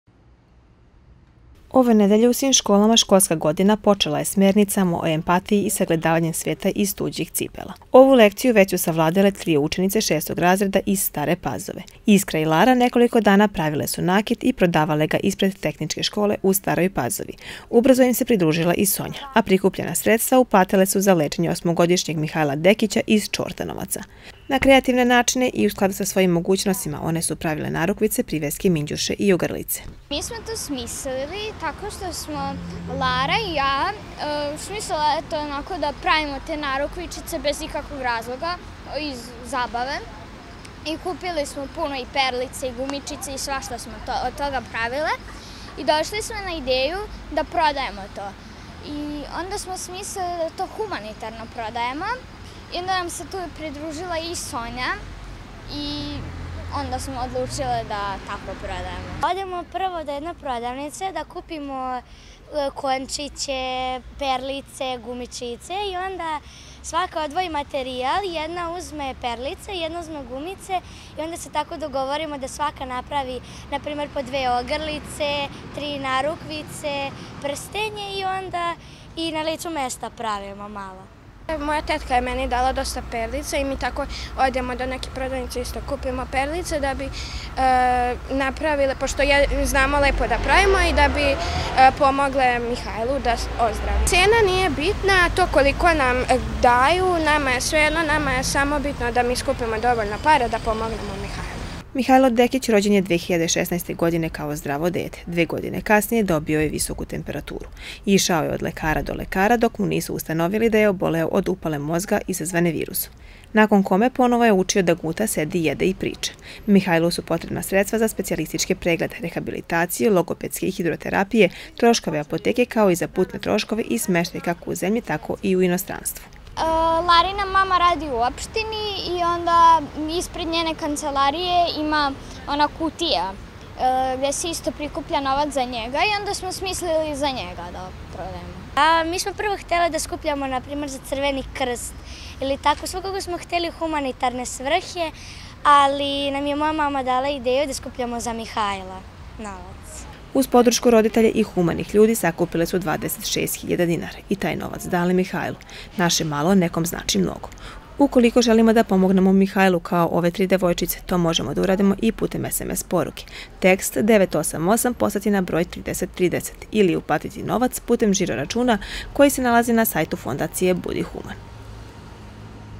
Dobročinstvo od malih nogu
Tokom prošle nedelje Iskra, Lara I Sonja ispred Tehničke škole u Staroj Pazovi prodavale su narukvice, ogrlice, priveske i minđuše kako bi sakupile novac za lečenje osmogodišnjeg Mihajla Dekića iz Čortanovaca. Prikupile su  26.000 dinara, a novac su uplatile MIhajlu